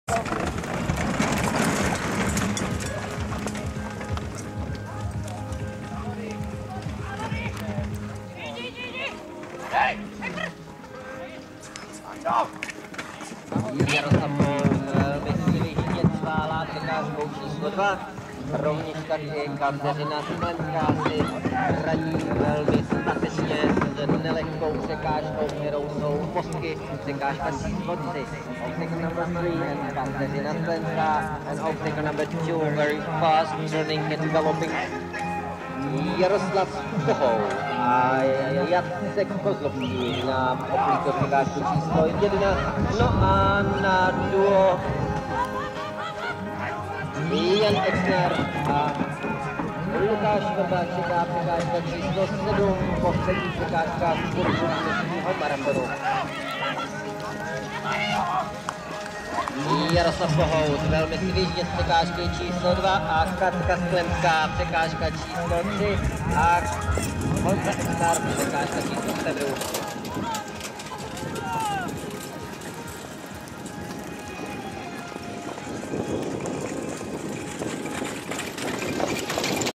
Sklenská Kateřina - Mezinárodní závody spřežení CAI3* Němčice 2015
Ve dnech 14. – 17. května proběhl ve středočeských Němčicích již druhý ročník mezinárodních závodů spřežení vypsaných hned v několika různých kategoriích. Kamera internetové televize EquiTV u této události samozřejmě nemohla chybět…

Dříve, než vám přineseme souhrnnou video reportáž, můžete se podívat na několik samostatných videí z nejatraktivnější části závodů – sobotního vozatajského maratonu.